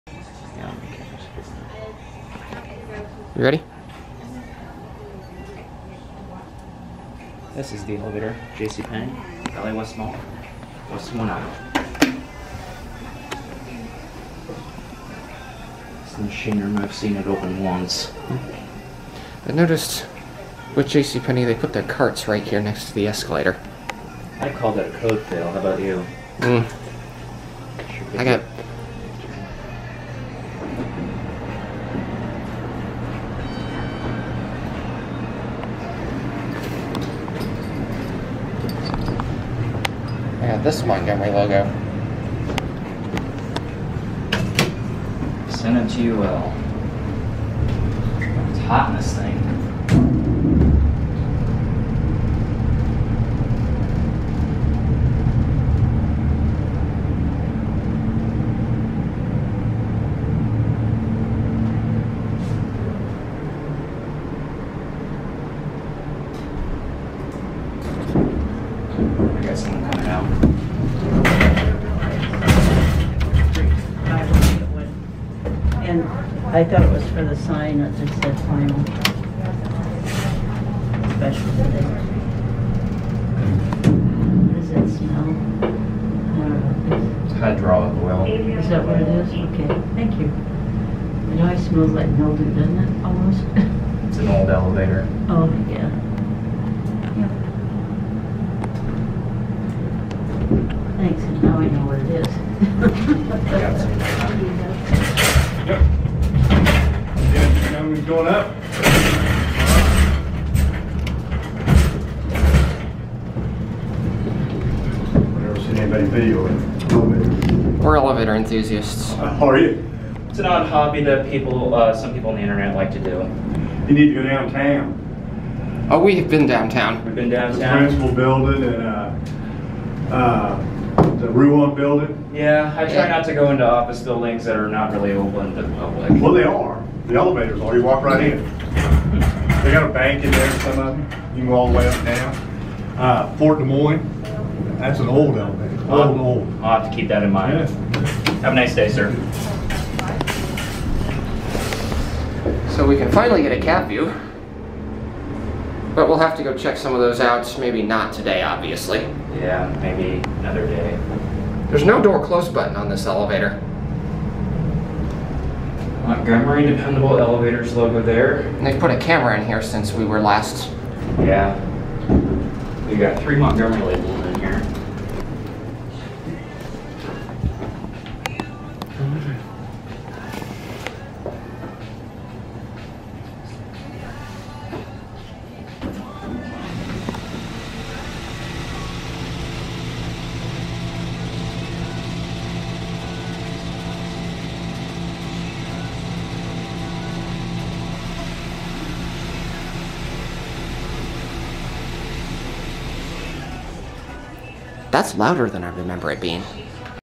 Montgomery hydraulic elevator at JcPenney, Valley West Mall, W. Des Moines IA
Shot 8/28/2021.  Featuring Super Techno Cat.

Just figured we needed to film this while we were here.  We both suspected at the time JcPenney may close soon after Von Maur, but to this day, the store is still open.